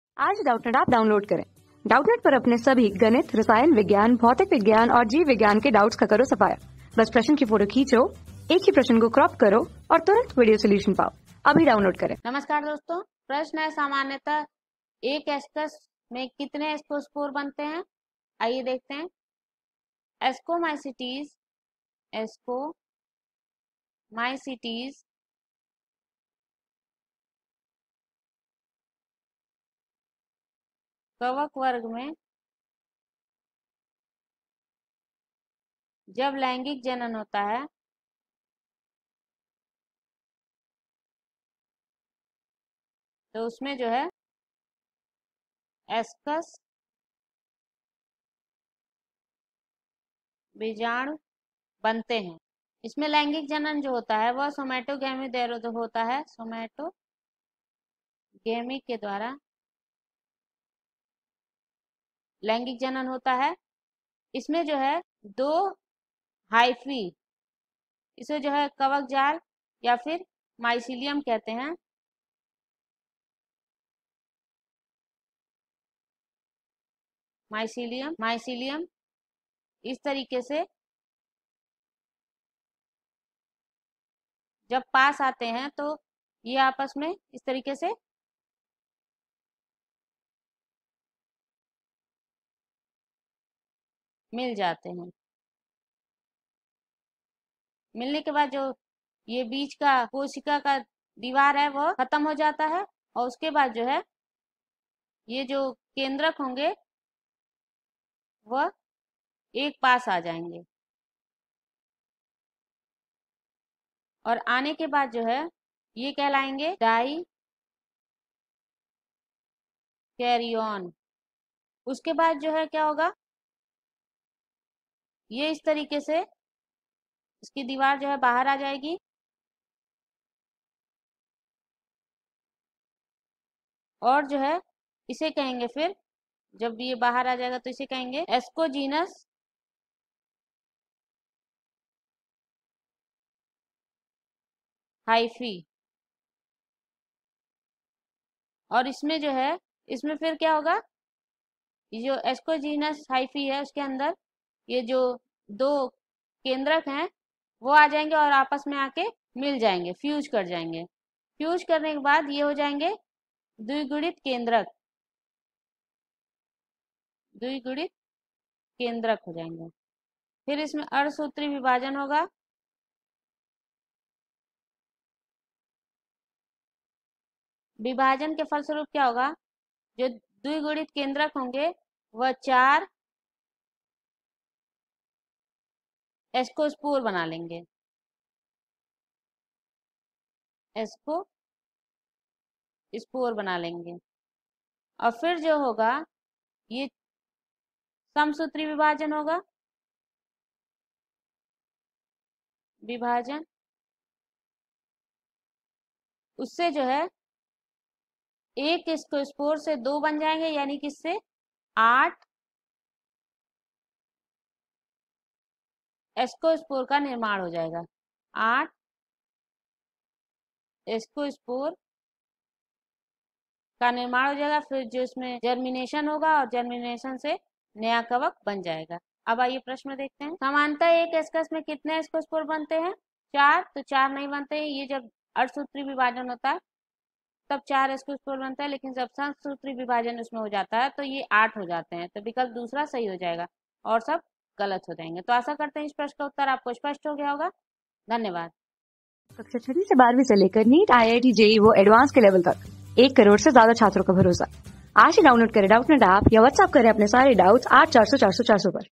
सामान्यतयाः एक एस्कस में कितने एस्कोस्पोर्स बनतें है? | 12 | PLANT DIVERSITY | BIOLOGY | ALLEN H...
सामान्यतयाः एक एस्कस में कितने एस्कोस्पोर्स बनतें है?
Class: 12
Subject: BIOLOGY
Chapter: PLANT DIVERSITY
Board:IIT JEE

👉 Have Any Query? Ask Us.